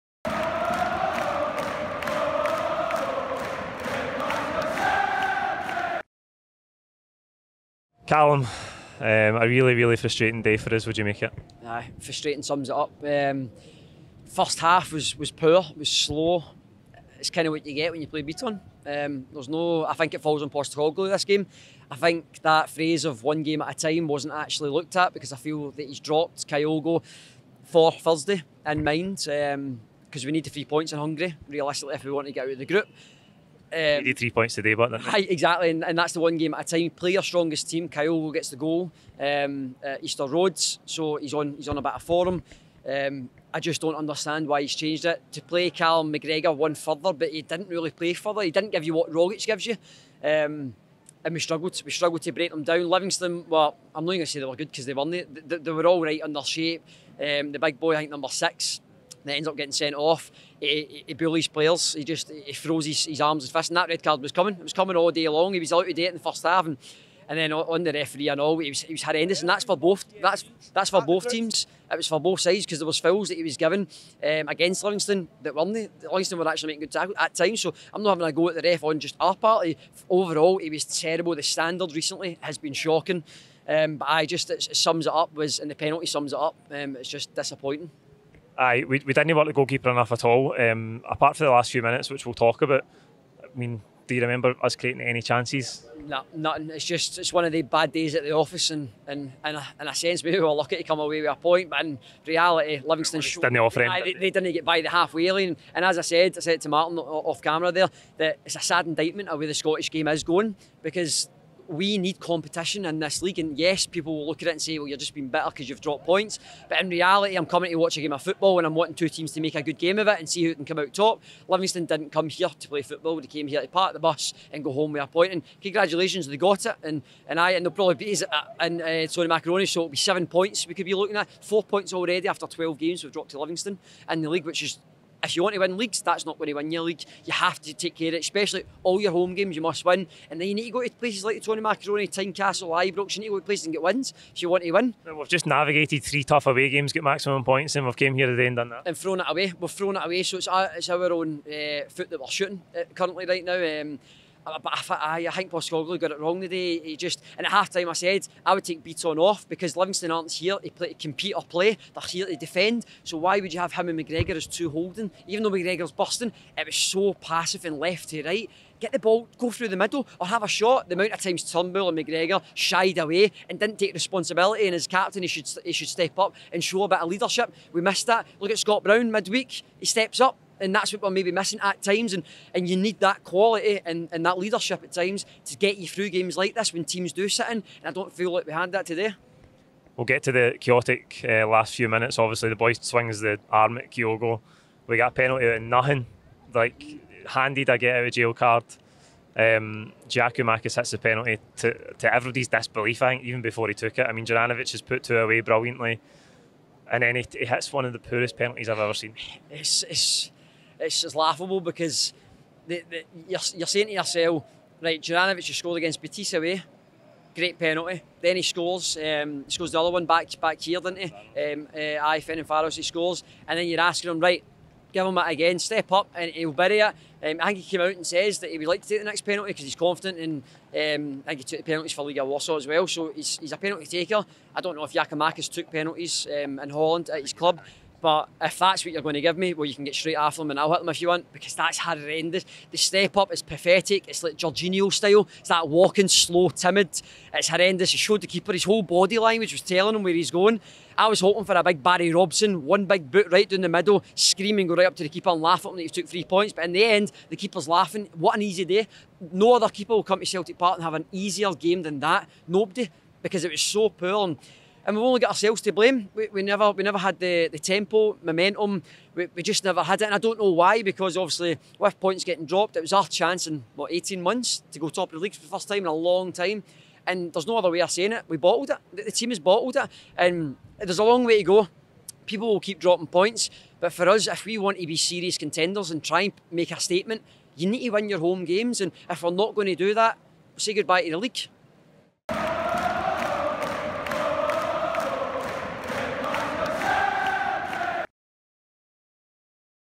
Celtic 0-0 Livingston | 'We Bottled It' | Full-Time Reaction
FOLLOW us across all social media platforms 👇🏻🍀